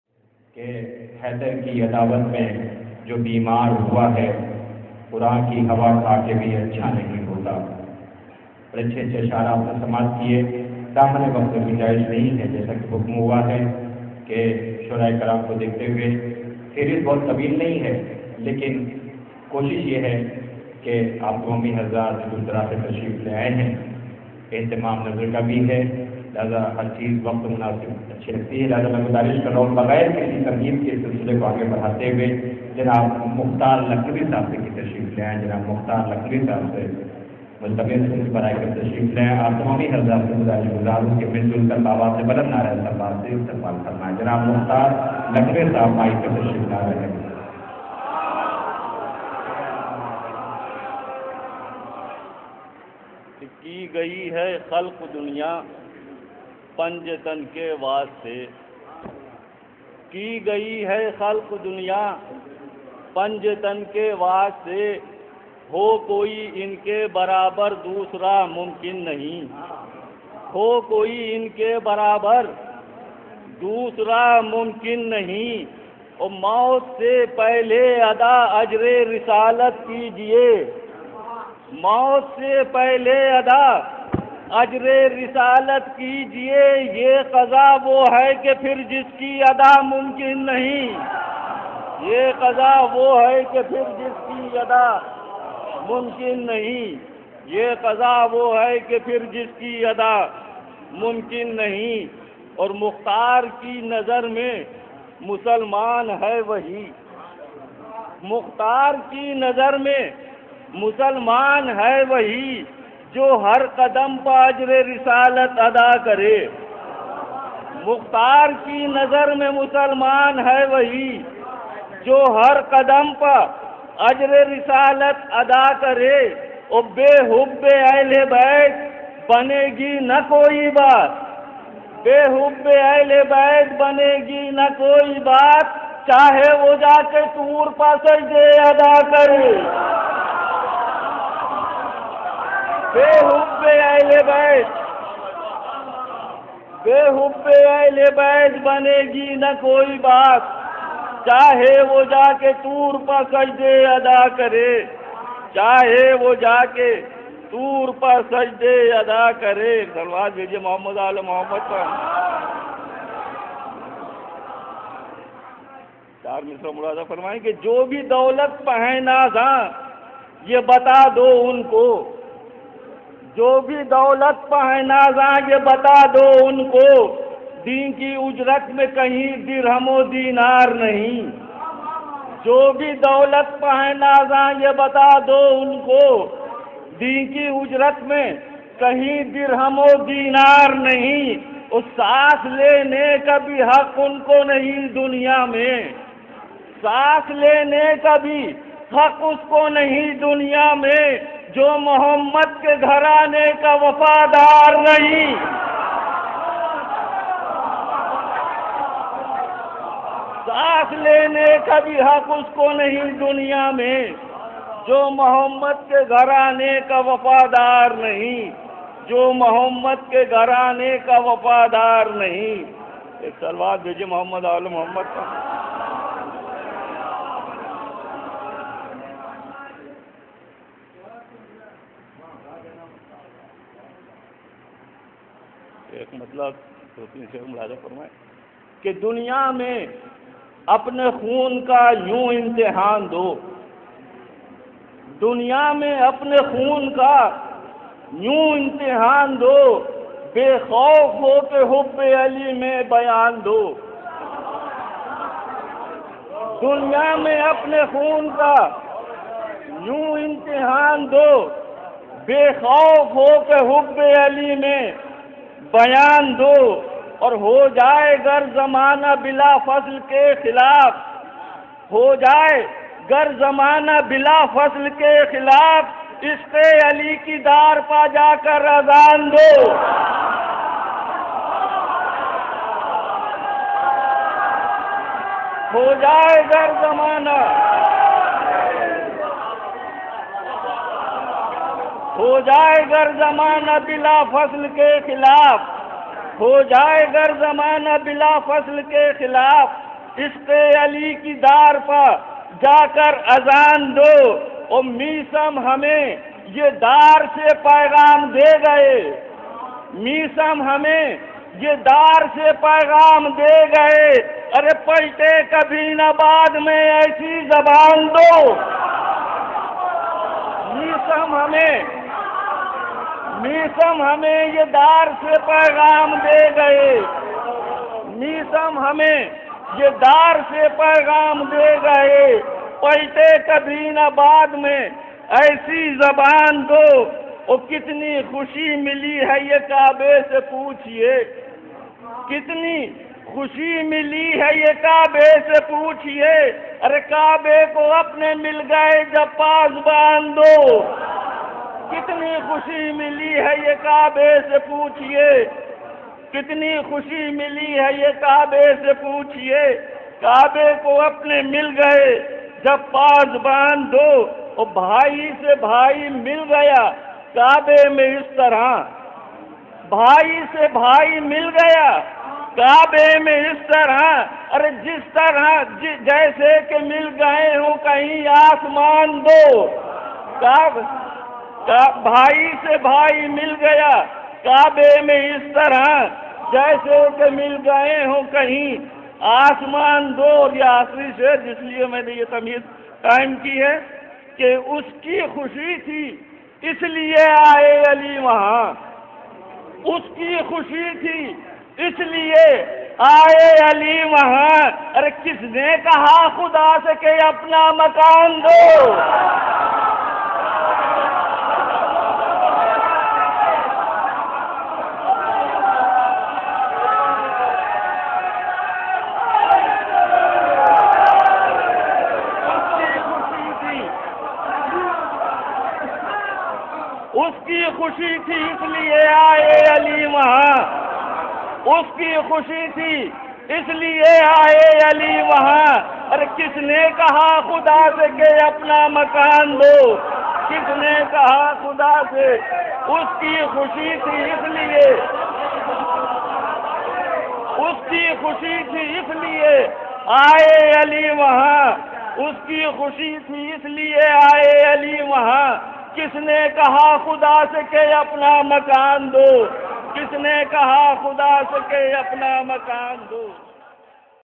JASHN-E-MAULOOD-E-HARAM 2013 By GRAFH AGENCY Lko. MUKHTAR LUCKNOWI
JASHN-E-MAULOOD-E-HARAM 2013. On 12 RAJAB 1434 Hijri At IMAMBARA AGHA BAQIR, Lucknow. Organized By : IDARA-E-KHUDDAM-E-MAULOOD-E-HARAM, Lucknow.